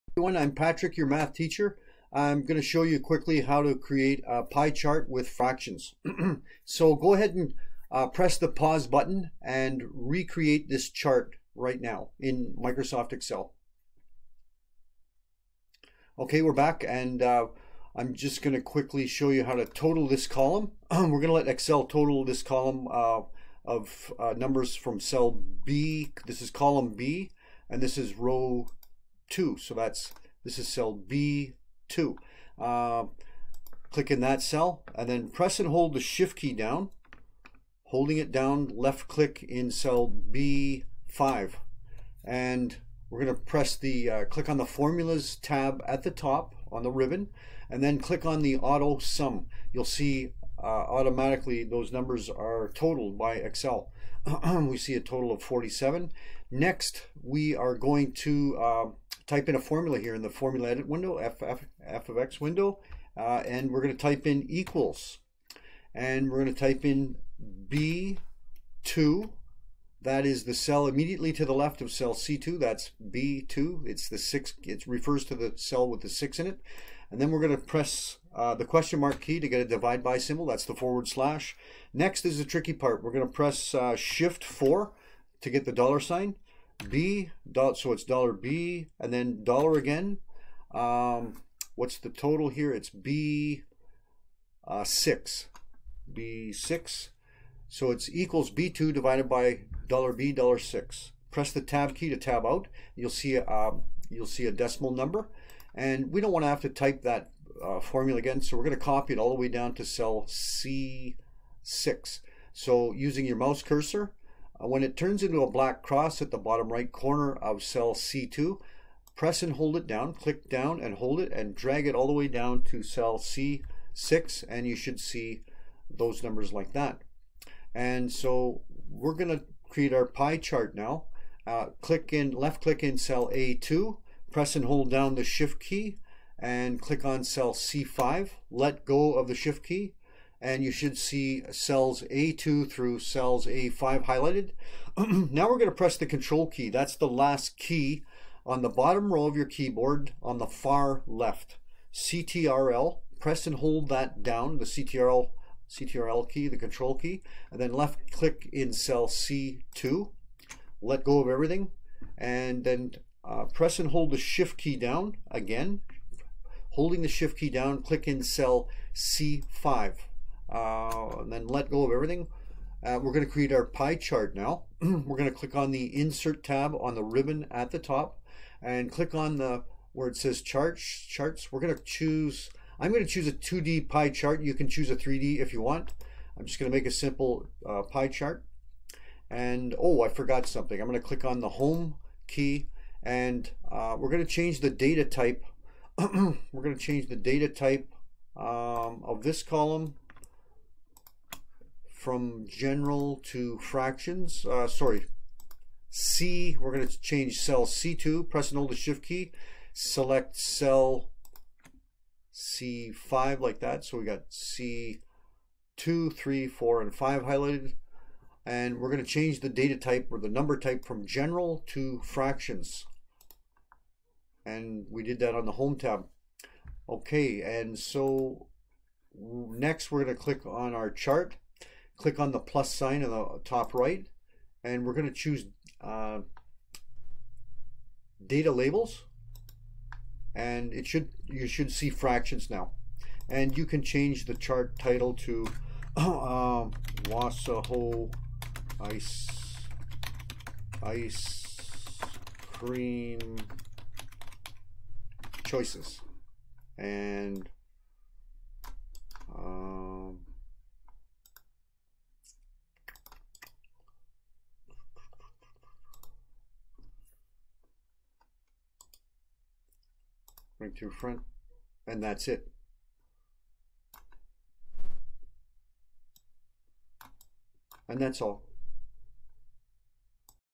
Creating a Pie Chart with Fractions in Excel
How to create a pie chart displaying fractions of a whole.